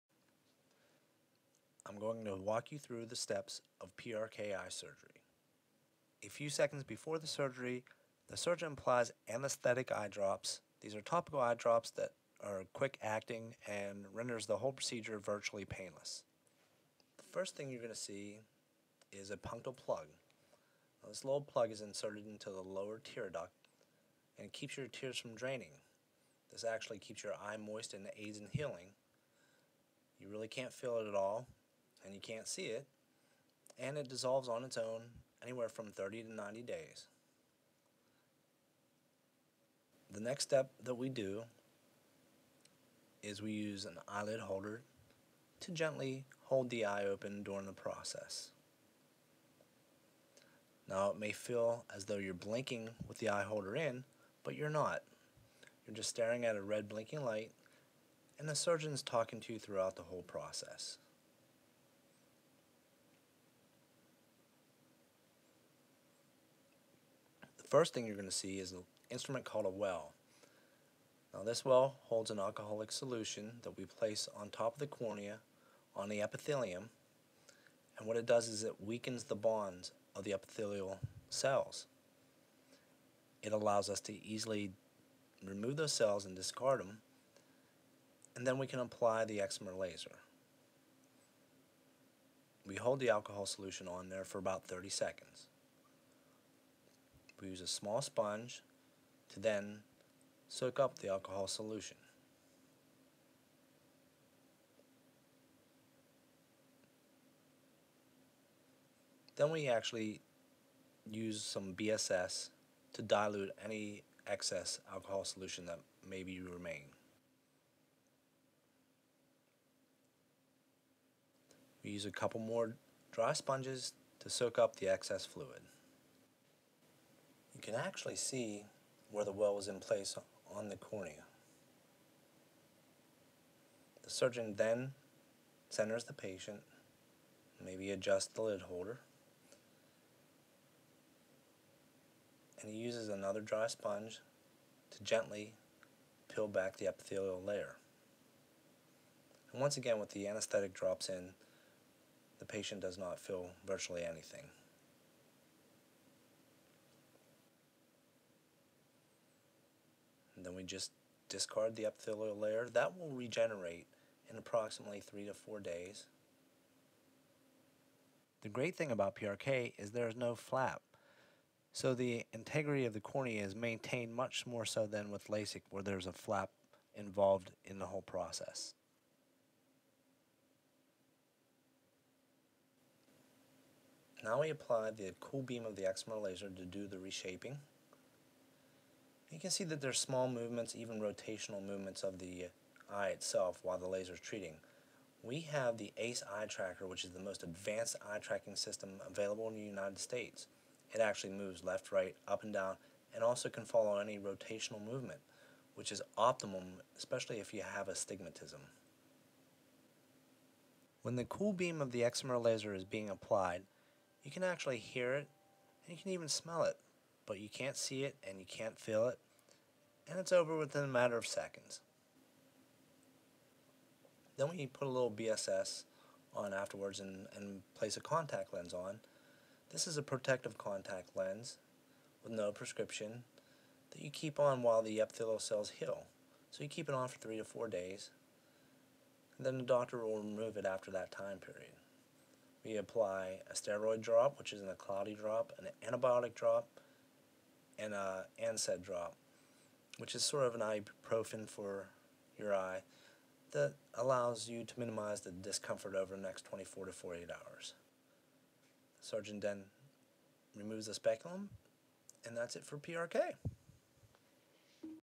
PRK Eye Surgery
Prk eye surgery instead of LASIK walks you through the entire Photo Refractive Keratoectomyy procedure.